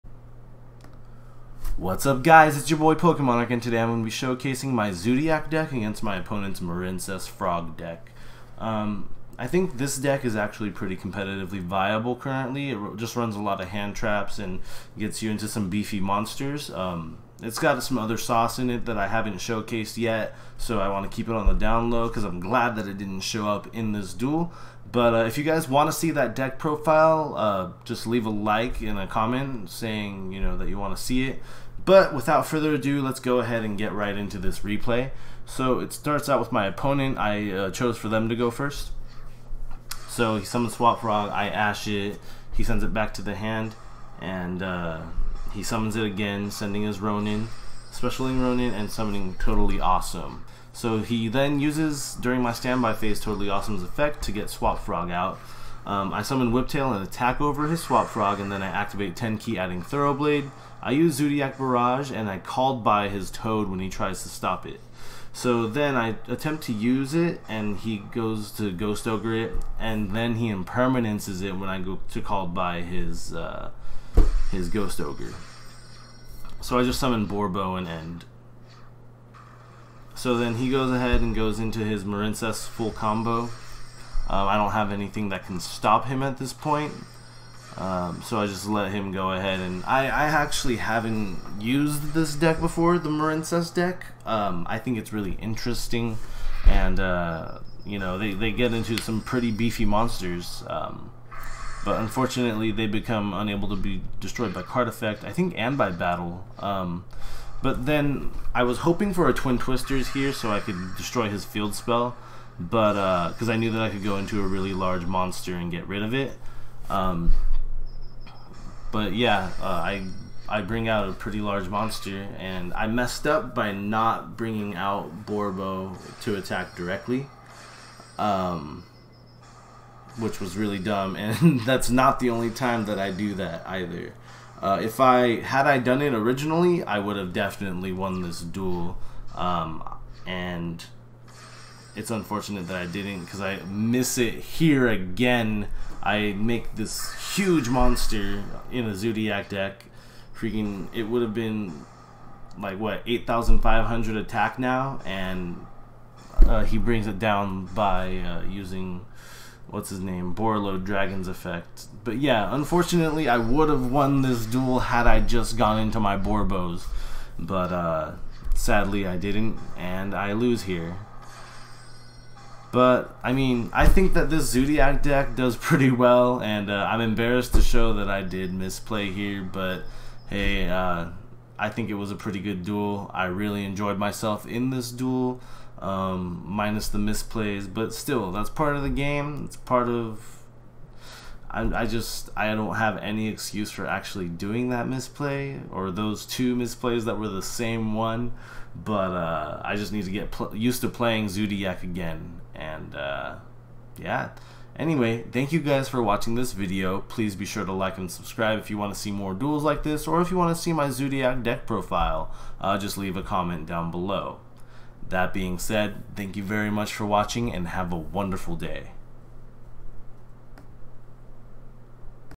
Yu-Gi-Oh! Zoodiac vs Marincess! UGH! The Misplay Madness!!!!!!
Hey guys, here is my Zoodiac deck vs my opponents Marincess'. Please be sure to leave a like on the video, and comment for me your worst misplays, because honestly... this is the worst misplay game i have had to date. LOL hope you get a good laugh!